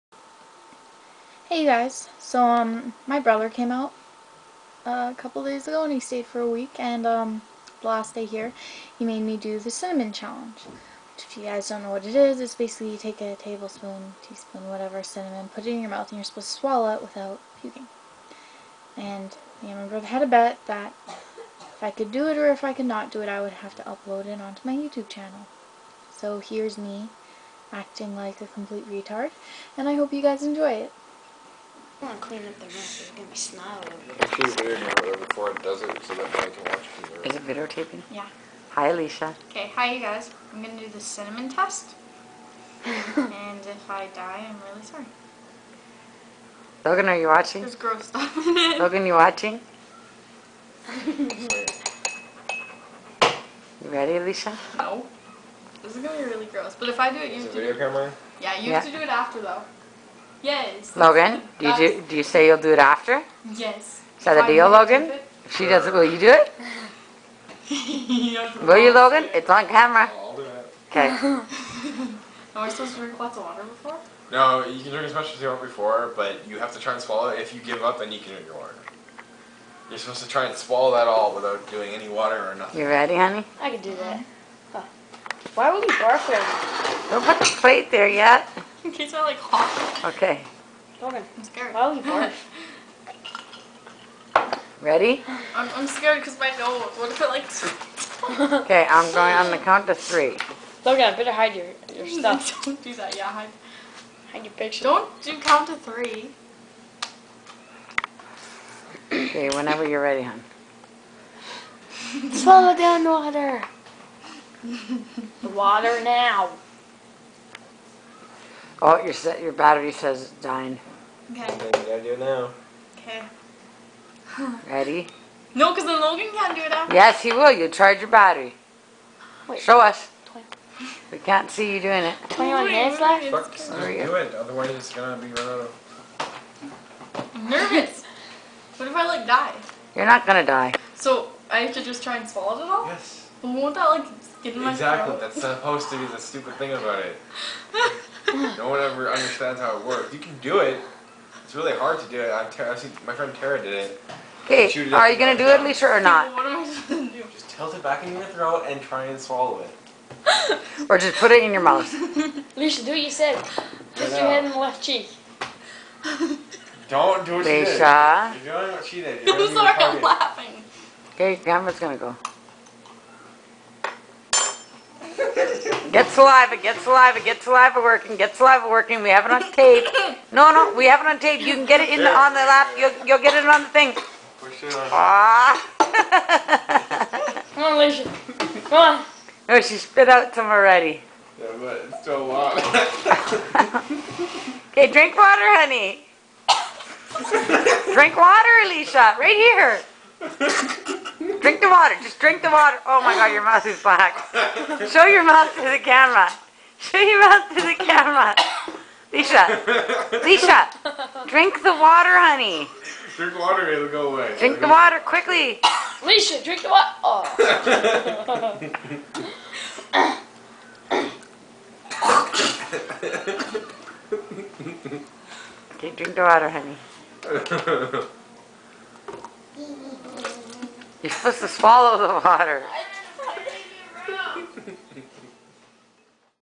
Me doing the cinnamon challenge lol
my brother dared me to do the cinnamon challenge and put it on my channel
(i know i look like a weirdo)

I DARE ALL OF YOU TO DO THE CINNAMON CHALLENGE